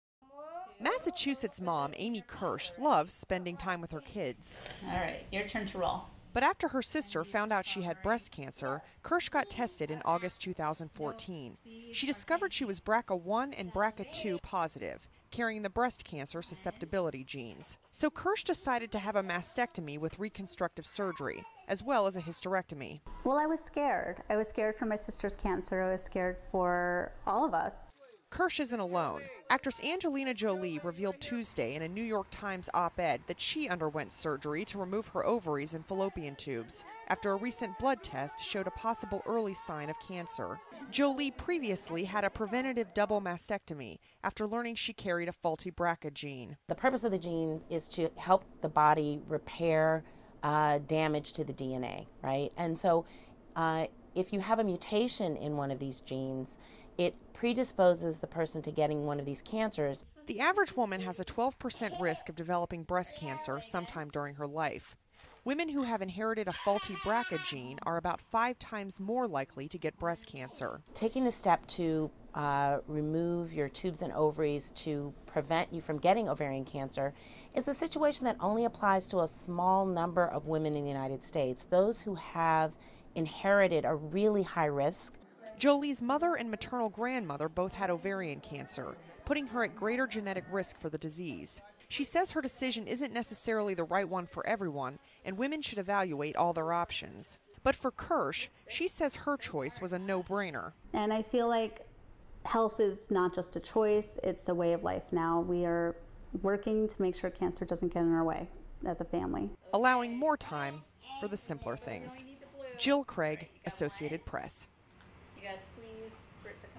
Oscar-winning actress Angelina Jolie revealed Tuesday that she has undergone more preventive surgery
(24 Mar 2015) FOR CLEAN VERSION SEE STORY NUMBER: apus028304

VOICE-OVER:
MASSACHUSETTS MOM AMY KIRSCH LOVES SPENDING TIME WITH HER KIDS.
(NATS - "Alright, your turn to roll")
BUT AFTER HER SISTER FOUND OUT SHE HAD BREAST CANCER, KIRSCH GOT TESTED IN AUGUST 2014. SHE DISCOVERED SHE WAS BRCA1 AND BRCA2 POSITIVE - CARRYING THE BREAST CANCER SUSCEPTIBILITY GENES.
SO KIRSCH DECIDED TO HAVE A MASTECTOMY WITH RECONSTRUCTIVE SURGERY, AS WELL AS A HYSTERECTOMY.
SOUNDBITE (ENGLISH) Amy Kirsch, diagnosed with BRCA1 and BRCA2 in August 2014; had both a mastectomy with reconstructive surgery and a hysterectomy:
"Well, I was scared. I was scared for my sister's cancer, I was scared for all of us."
KIRSCH ISN'T ALONE.
ACTRESS ANGELINA JOLIE REVEALED TUESDAY IN A NEW YORK TIMES OP-ED THAT SHE UNDERWENT SURGERY TO REMOVE HER OVARIES AND FALLOPIAN TUBES, AFTER A RECENT BLOOD TEST SHOWED A POSSIBLE EARLY SIGN OF CANCER.
JOLIE PREVIOUSLY HAD A PREVENTATIVE DOUBLE MASTECTOMY, AFTER LEARNING SHE CARRIED A FAULTY BRCA GENE.
SOUNDBITE (ENGLISH) Carol Brown, MD, gynecologic oncologist, Memorial Sloan Kettering Cancer Center:
"The purpose of the gene is to help the body repair damage to the DNA. And so, if you have a mutation in one of these genes it predisposes the person to getting one of these cancers."
THE AVERAGE WOMAN HAS A 12 PERCENT RISK OF DEVELOPING BREAST CANCER SOMETIME DURING HER LIFE. WOMEN WHO HAVE INHERITED A FAULTY BRCA GENE ARE ABOUT FIVE TIMES MORE LIKELY TO GET BREAST CANCER.
"Taking a step to remove your tubes and ovaries to prevent you from getting ovarian cancer is a situation that only applies to a small number in the United States. Those who have inherited a really high risk."
JOLIE'S MOTHER AND MATERNAL GRANDMOTHER BOTH HAD OVARIAN CANCER, PUTTING HER AT GREATER GENETIC RISK FOR THE DISEASE.
SHE SAYS HER DECISION ISN'T NECESSARILY THE RIGHT ONE FOR EVERYONE AND WOMEN SHOULD EVALUATE ALL THEIR OPTIONS.
BUT FOR KIRSCH, SHE SAYS HER CHOICE WAS A 'NO-BRAINER.'
"I feel like health is not just a choice, it's a way of life now. We are working to make sure cancer doesn't get in our way."
ALLOWING MORE TIME FOR THE SIMPLER THINGS.
JILL CRAIG, Associated Press